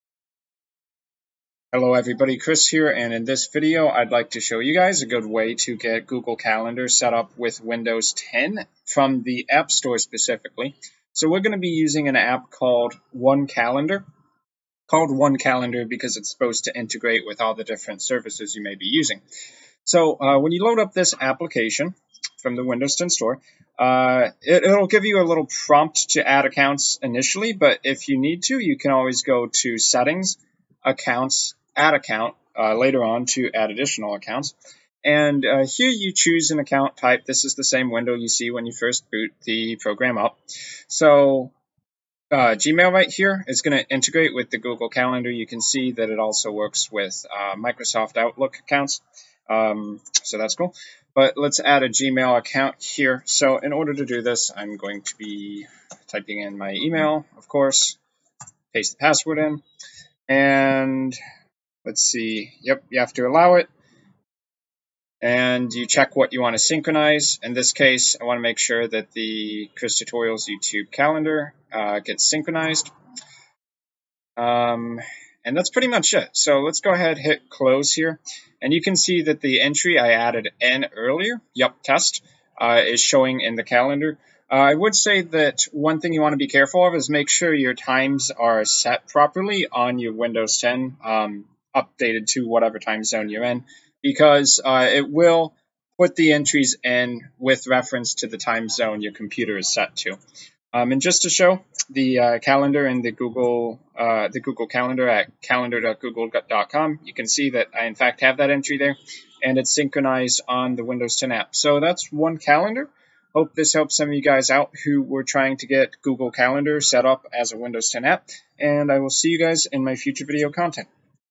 How to Get ALL Your Google Calendars in Windows 10 App | One Calendar Introduction
Good solution for integrating all Google Calendars with Windows 10 apps. Advantage to One Calendar is that it will properly bring in all calendars associated with your Google account.

Slightly Harder Alternative: Add Google Account to Outlook.com and use the Official Windows 10 App

Bitcoin: 17Pn5PifFmRkHRAqCZQAom1ZjCiSf7qJjJ
Ethereum: 0x6f2781F382952c8caCDbE99C46F07c265ab59627